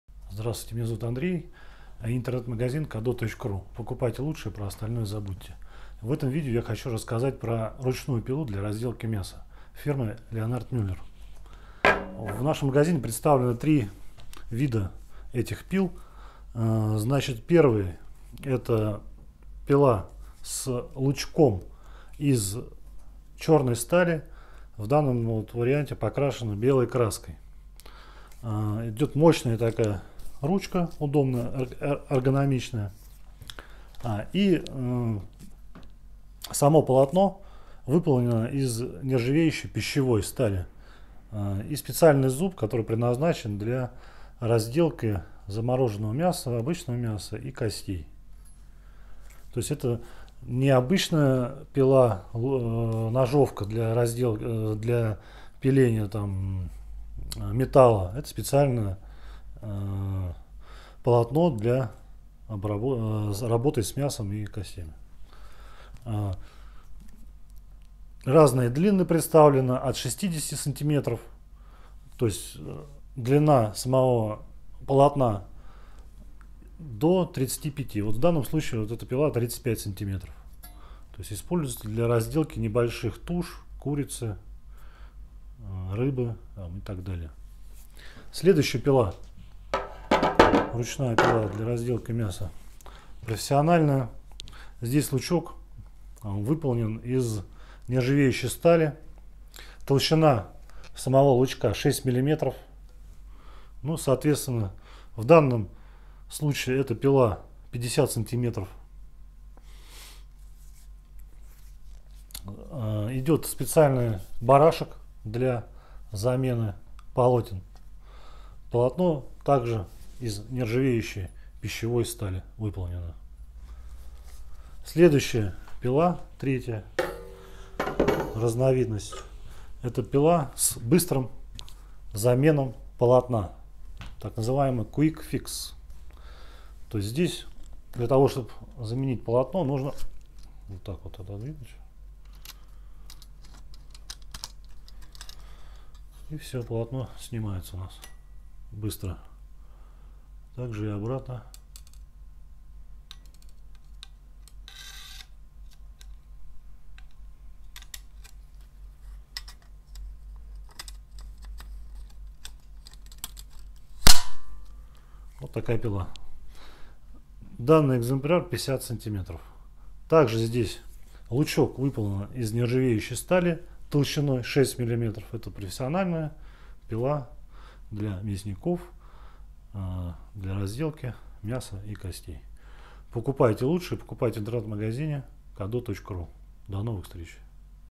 Ручная пила для мяса и костей Leonhard Mueller - купить пила для мяса ручная в интернет-магазине
Обзор ручных пил для мяса и костей австрийского производителя Leonhard Mueller&
Толщина лучка 6 мм.
Полотно из нержавеющей стали в комплекте.
Длины полотен: 350, 400, 450, 500, 600 мм

0:09 ручные пилы для разделки мяса
0:24 пила для мяса с лучком из черной стали
01:34 ручная пила для разделки мяса лучком из нержавеющей стали
02:09 профессиональная ручная пила для мяса с быстрой заменой полотна и лучком из нержавейки